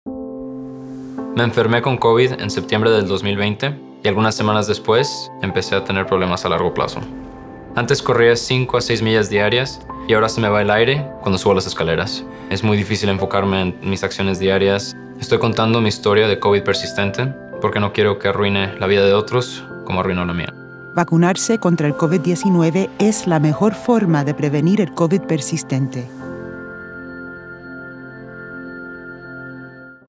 Voces del COVID Prolongado - Rob (:30)
Cualquiera puede sufrir del COVID Prolongado. Rob comparte su trayectoria con el COVID Prolongado.